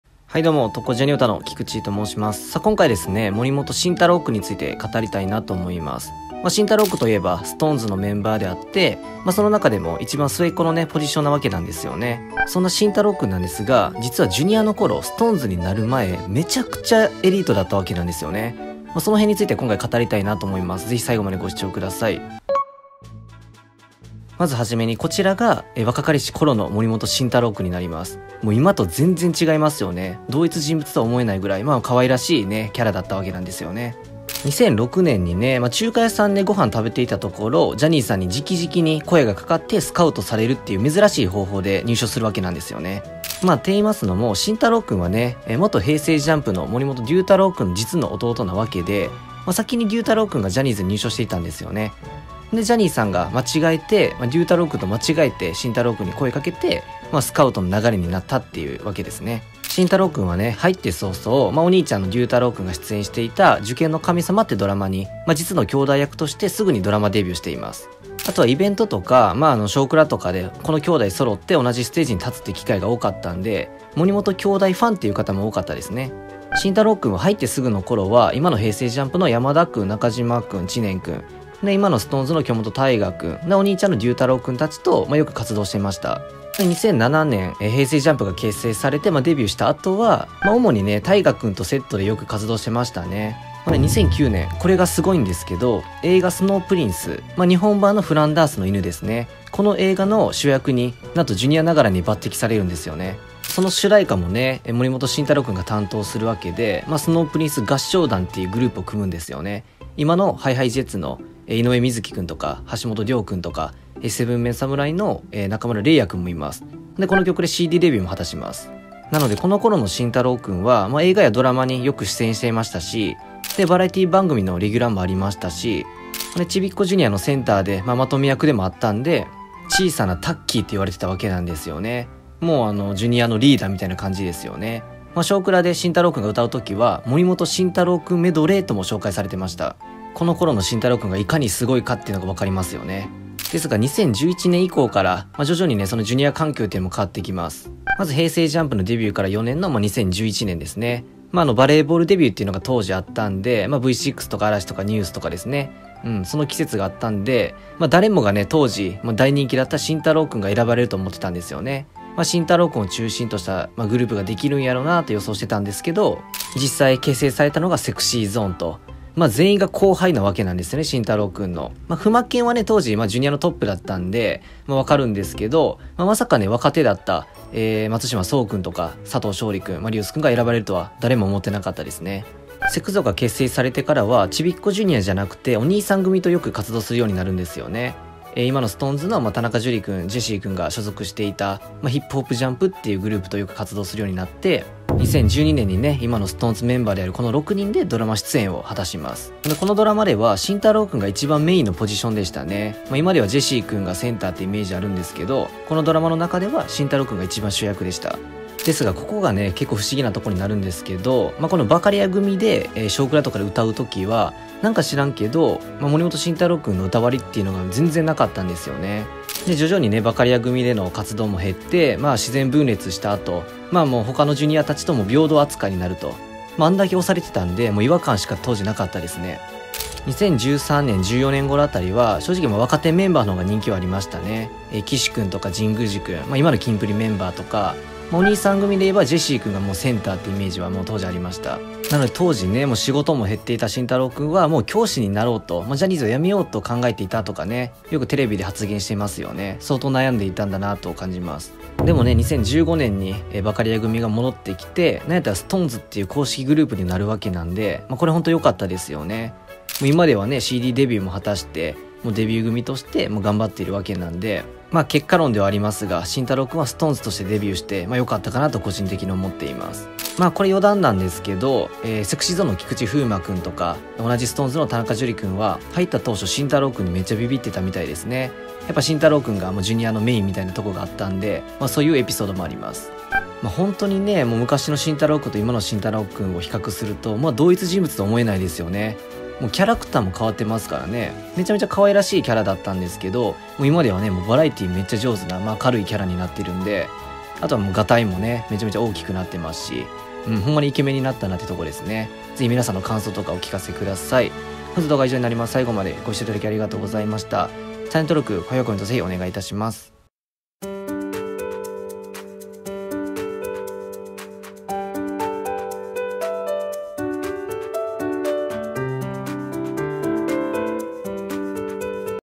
【SixTONES】森本慎太郎の歴史をまとめてみた！
【SixTONES】について語ります！

※キクチー・エンタテイメントはジャニーズについてただただ語るチャンネルです！